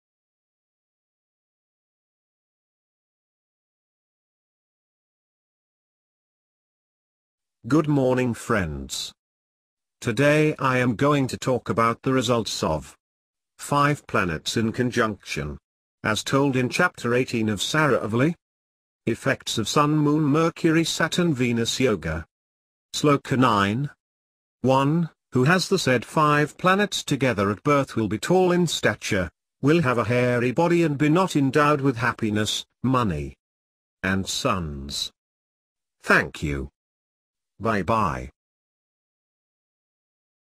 SUN MOON MERCURY SATURN VENUS YOGA
This Video talks about five Planets,SUN MOON MERCURY SATURN VENUS YOGA,  in the Ascendant and their effects as per chapter 18 of saravali.